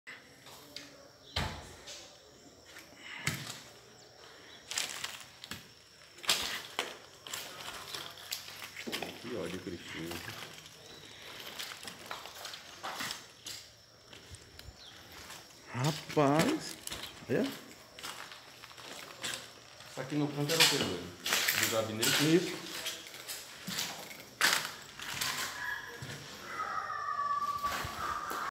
Fogo destrói prefeitura de São José da Vitória-Ba
Um incêndio destruiu o prédio da Prefeitura de São José da Vitória, no sul da Bahia, na madrugada desta sexta-feira (4). A perícia investiga as causas do incêndio ocorrido menos de 20 dias depois das eleições municipais. O prefeito Jeová Nunes (PSB) venceu a disputa. As informações iniciais são de que ninguém ficou ferido, apesar de imóveis vizinhos.Uma viatura de grande porte do Corpo de Bombeiros foi deslocada do Grupamento em Itabuna para debelar e controlar o incêndio ainda na madrugada desta sexta. Há suspeita de incêndio criminoso, pois teriam sido encontrados vasilhames com forte cheiro de combustível. Até que a perícia e as investigações apontem as causas e possíveis autores,muitas especulações seguirão povoando o imaginário do povo da cidade,que nunca precensiou algo parecido envolvendo o Paço Municipal.